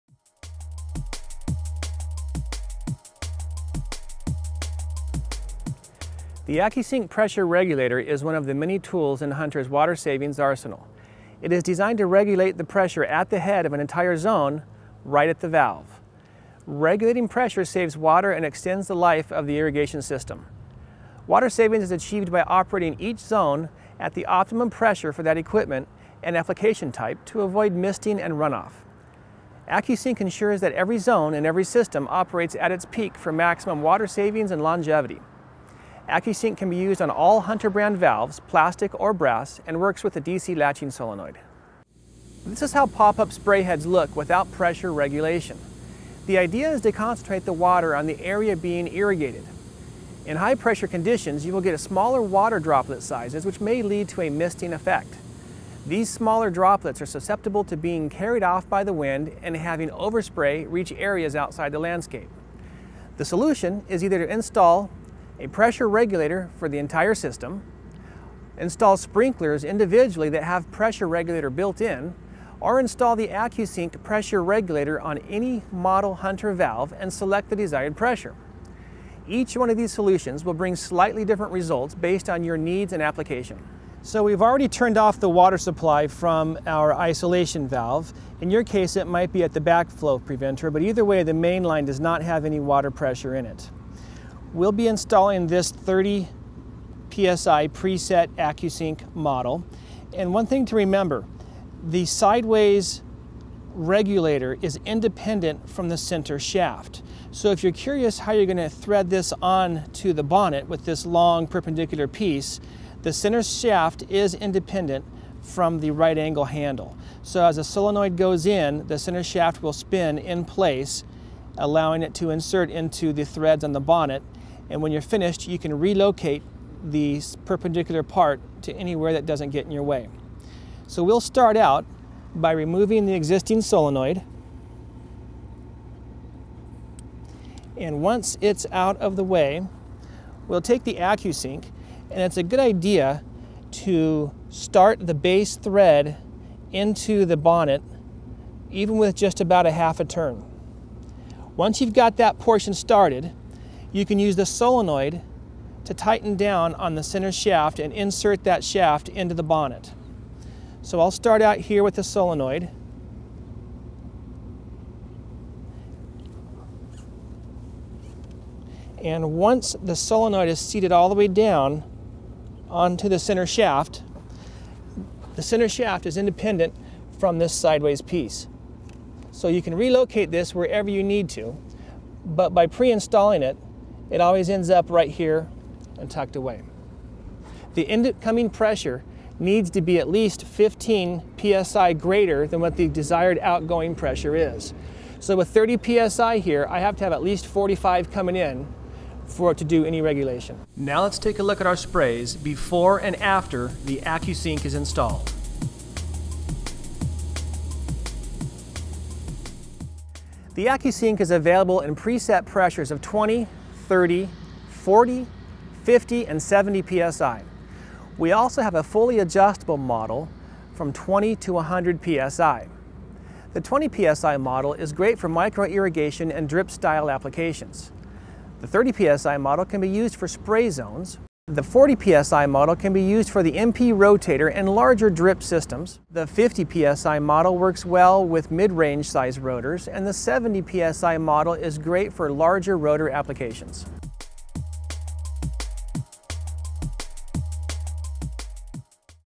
Accu-Sync Valve Pressure Regulator Installation, Adjustment and Overview
The Accu-Sync pressure regulator is designed to regulate the pressure at the valve for the entire zone to provide maximum water efficiency. Learn how to correctly install the Accu-Sync regulator on the solenoid inside of the valve box.